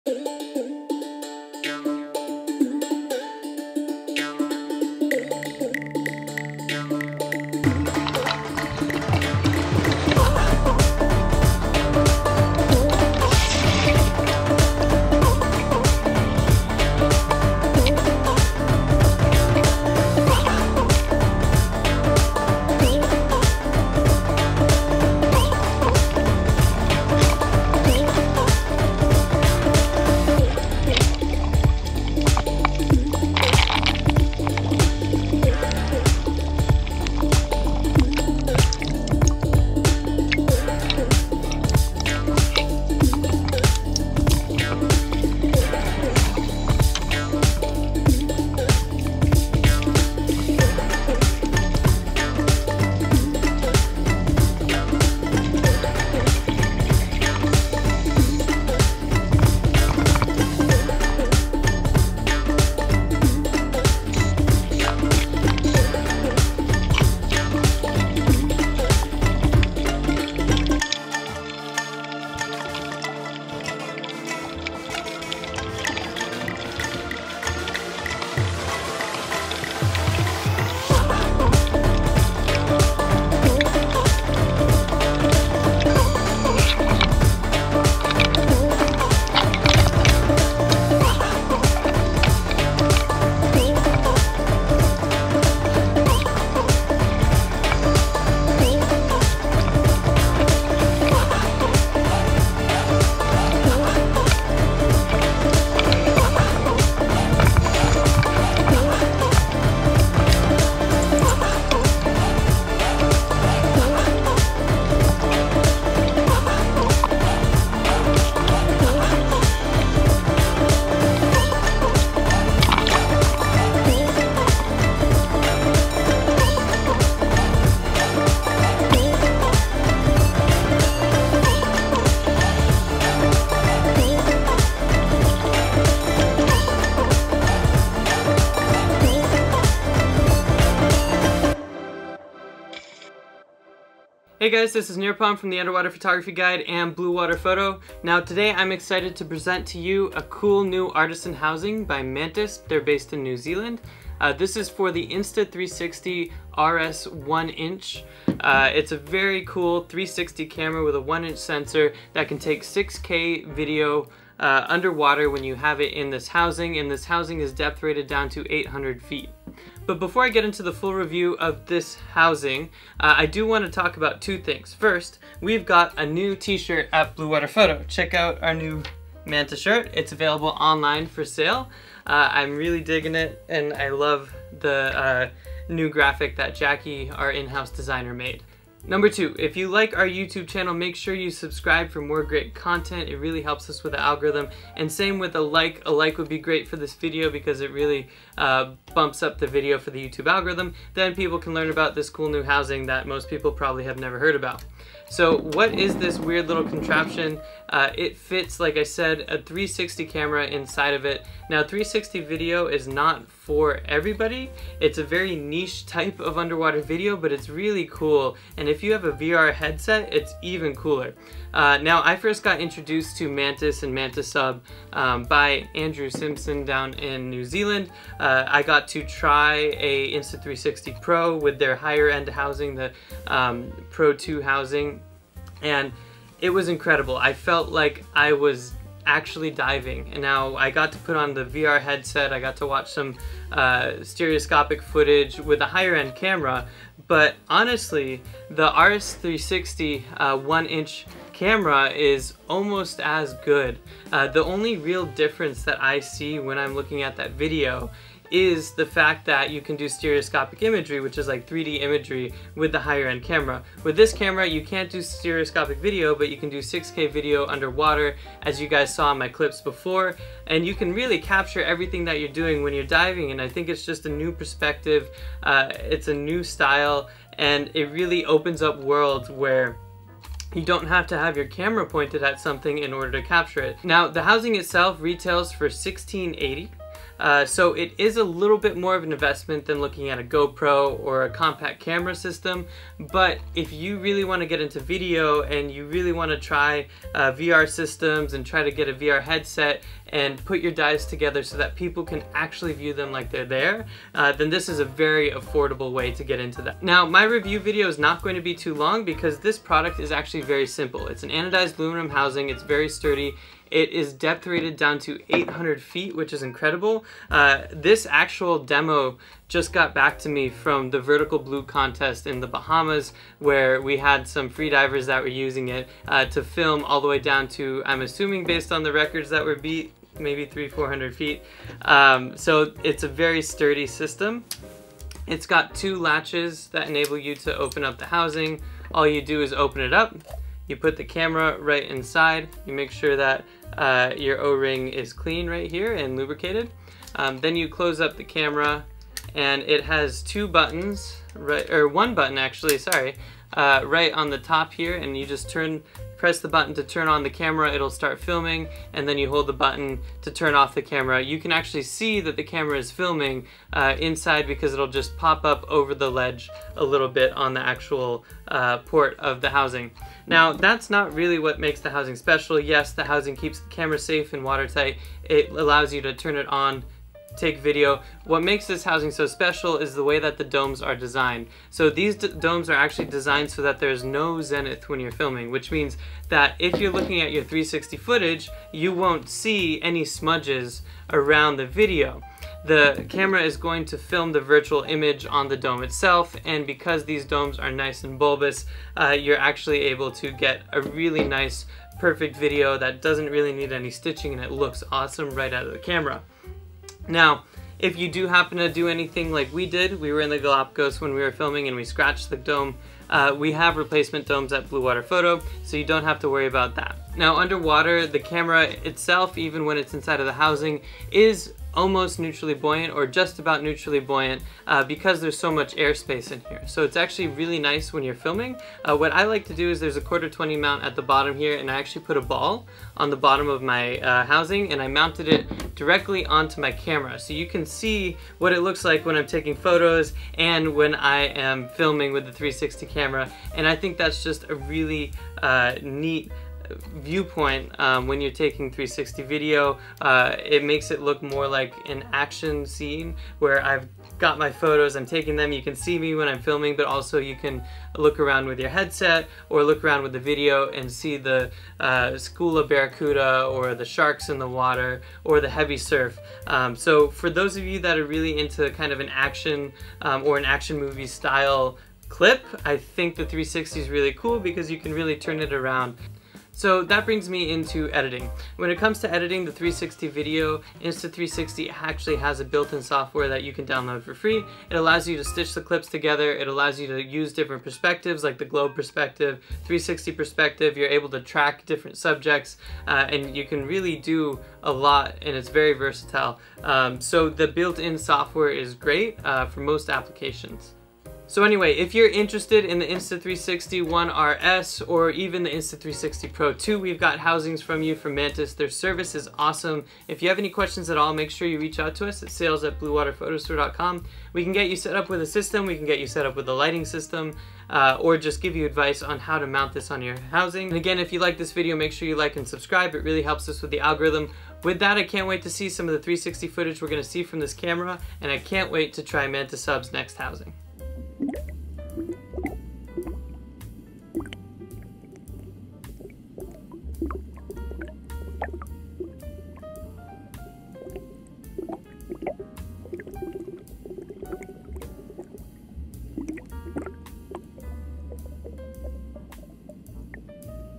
Insta360 One RS (Mantis Sub RS Housing) Underwater Review
If you're looking into shooting 360 underwater video, the Mantis Sub RS 360 housing is the perfect place to start!

The Insta360 One RS can take incredible 360 (6K) footage and the Mantis Sub RS 360 underwater housing is the best way to take the camera underwater. The housing is depth rated to 820ft and has a simple, easy-to-use one-press button design.

We took the housing and camera diving during our @bluewatertravel@Bluewaterdivetravel underwater photoworkshop to the Galapagos. As you can see from the footage of sealions, marine iguanas, and barracuda the results are incredible.

Jump to a Section:
0:00 - 360 underwater video
2:24 - Introduction
5:06 - Housing Features
7:56 - Editing 360 Video
8:20 - Conclusions